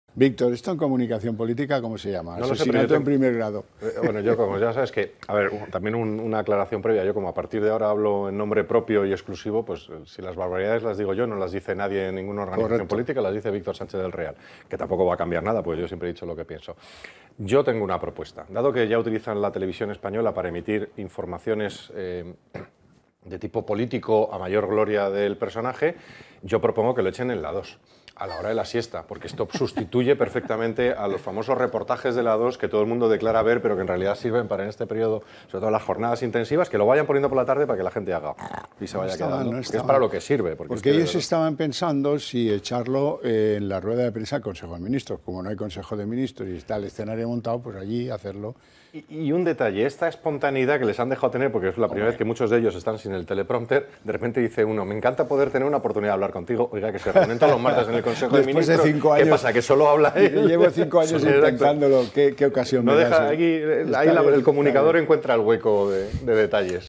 Víctor Sánchez del Real propone que el ‘show’ de Sánchez se emita en La 2… ¡por soporífero!
El espectáculo de autobombo de Pedro Sánchez en Ferraz con los aplausos de sus ministros de fondo tiene una posible salida televisiva: Víctor Sánchez del Real propone que se emita en el segundo canal de RTVE por una curiosa razón. Este nuevo perfil del presidente en el que se mete en el papel de entrevistador (y de entrevistado, porque el coloquio se convierte en monólogo) forma parte de la nueva estrategia de los socialistas para aupar la figura de su secretario general con el fin de volver a ganar las elecciones el próximo 23 de julio, algo que, a juzgar por los sondeos, está lejos de conseguir.